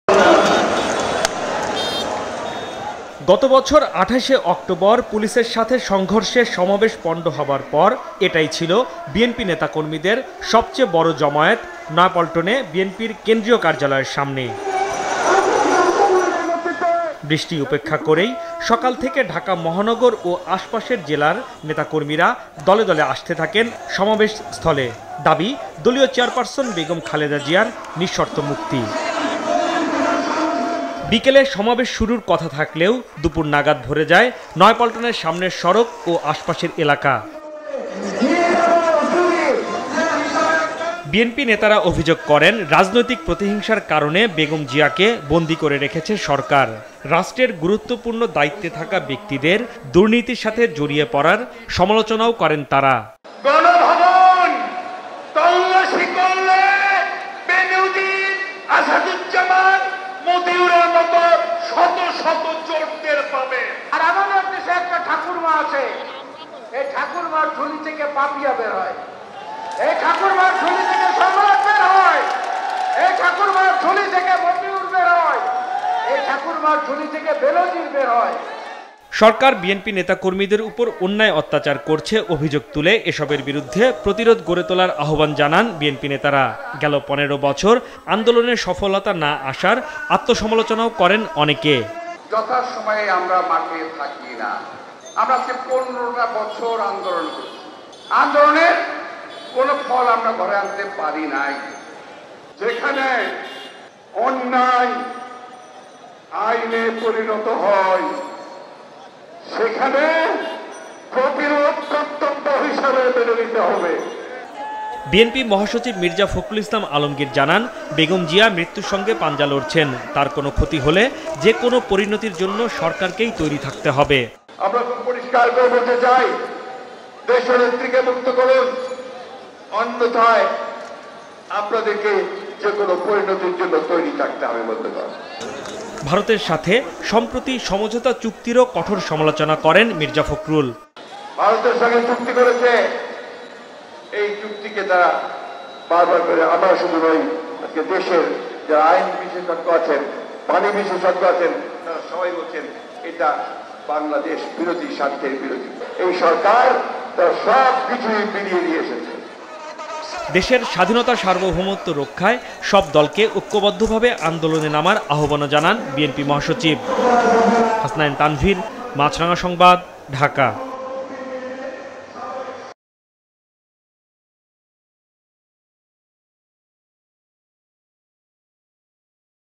খালেদা জিয়াকে মুক্তি না দিলে পরিণতির জন্য তৈরি থাকুন: ফখরুল | Maasranga News
খালেদা জিয়াকে মুক্তি না দিলে পরিণতির জন্য তৈরি থাকুন: ফখরুল

2 Bir Uttam Ziaur Rahman Road (New Airport Road)